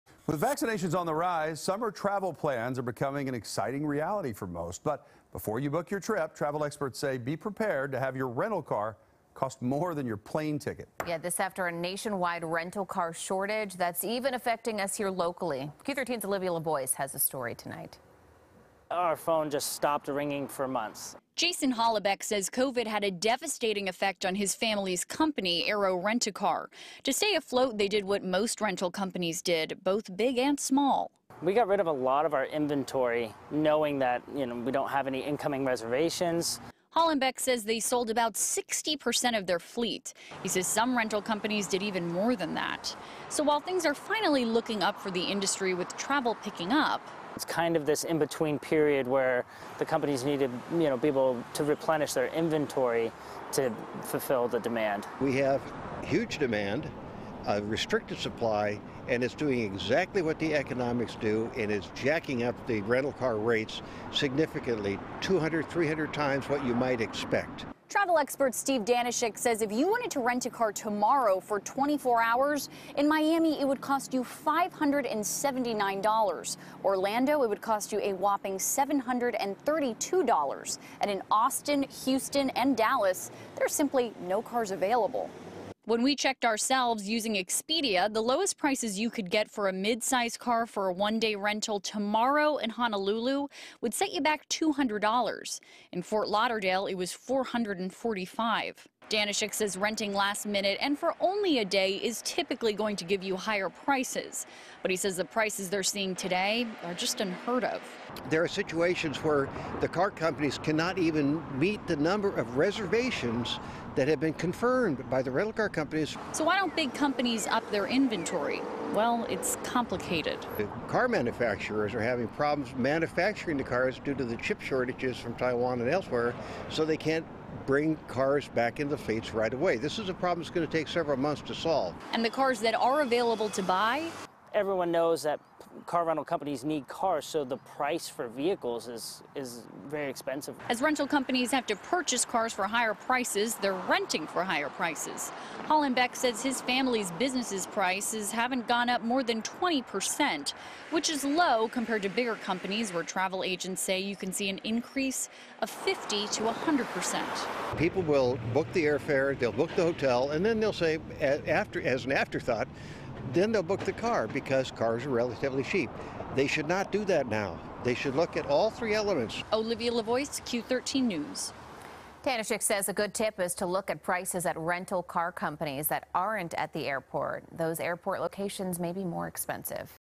Rental cars surge in price
Next time you travel your rental car could end up costing you more than your plane ticket.